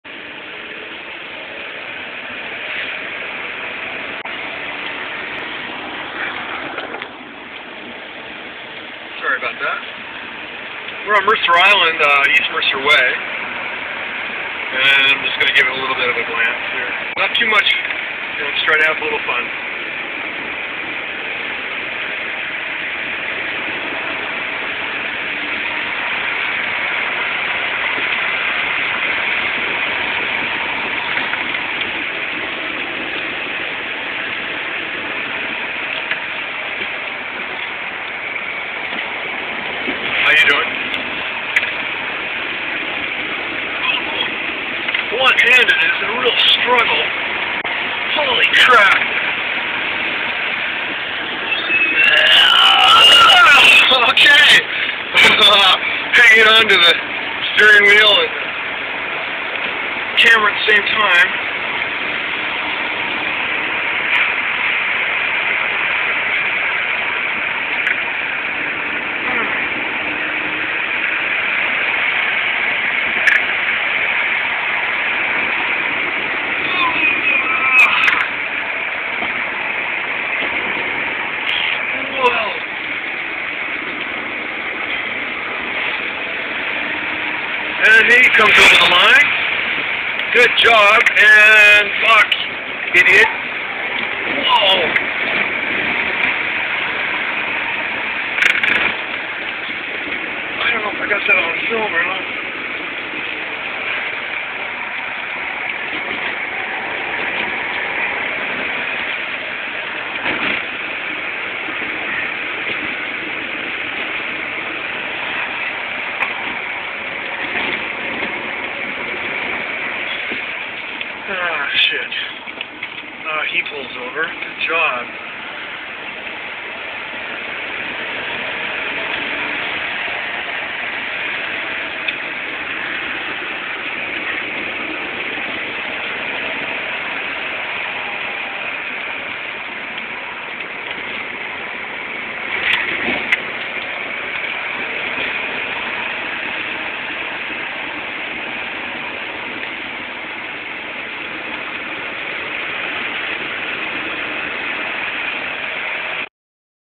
1977 Porsche 930 Turbo Carrera Mercer Island Run
My 1977 triple black 'Turbo Carrera'. Affectionately known to many throughout the PNW PCA as 'Turbo C'. East Mercer Way has always been an amazing road to carve whether in my 930 or on my 2000 Suzuki Hayabusa.

With my cell phone in my right hand to capture I didn't realize until AFTER I had recorded this that I had to tilt to sideways to capture correctly. My apologies. When I can correct it, I will.

Until then,, enjoy my sideways, one handed (more than a handful with 930 back and forth brutal steering) clip in the 930. I never got out of second gear. hehe